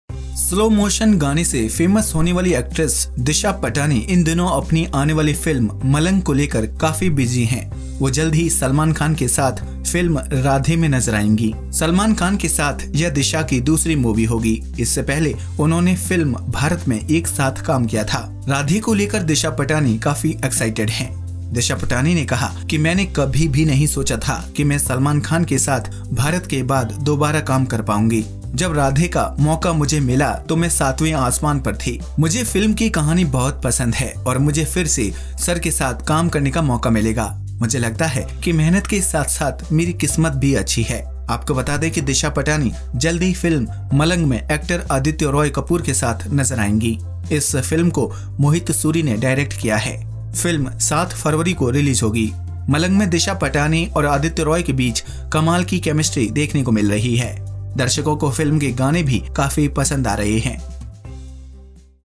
अपने दम पर फिल्में कराती है हिट, आएगी राधे में नजर|Disha Patani on working with Salman Khan in Radhe
Akshvi News